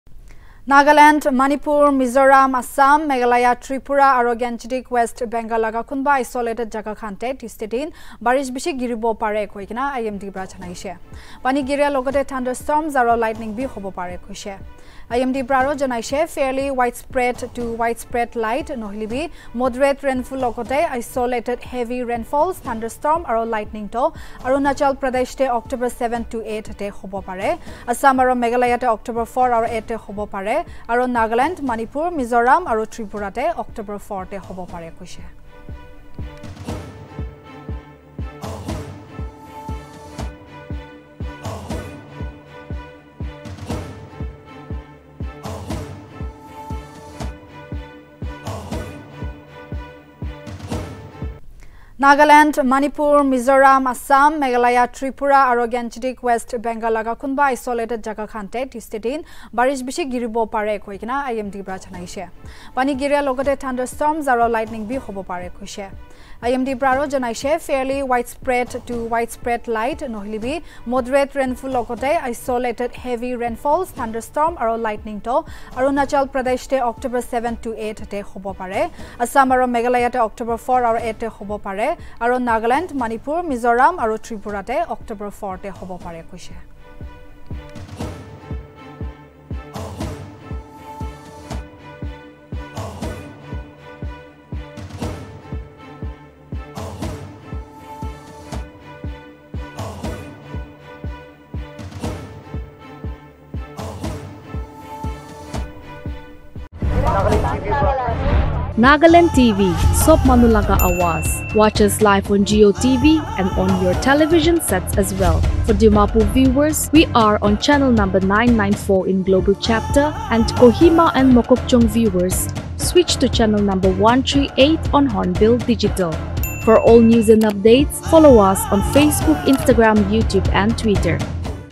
IMD PREDICTS HEAVY RAINFALL IN NAGALAND & OTHER NE STATES
The Indian Meteorological Department has predicted rainfall in the northeastern part of India from October 4 onwards. According to IMD, fairly widespread to widespread light or moderate rainfall with isolated heavy rainfall is very likely for Assam, Meghalaya, Nagaland, Manipur, Mizoram and Tripura on October 4. IMD stated that fairly widespread to widespread light or moderate rainfall with isolated heavy falls accompanied by thunderstorms and lightning is very likely over Arunachal Pradesh from October 5 to 7, Assam and Meghalaya on October 4 and 7, and over Nagaland, Manipur, Mizoram and Tripura on October 4